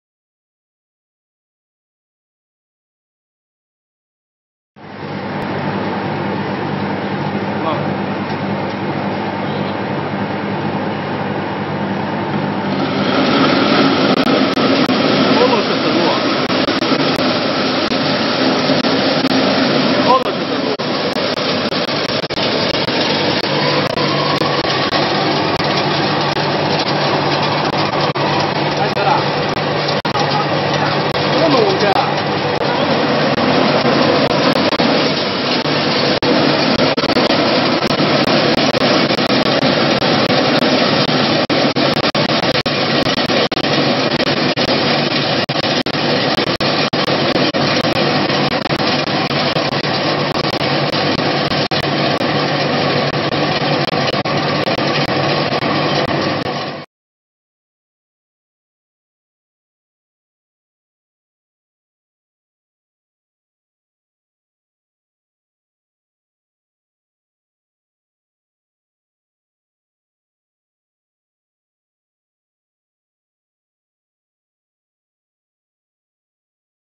Станок для очистки сои от оболочки JXDP-300
Предлагаем вашему вниманию видео работы станка для очистки оболочки сои JXDP-300. С полным описанием и характеристиками вы можете ознакомиться, посетив наш сайт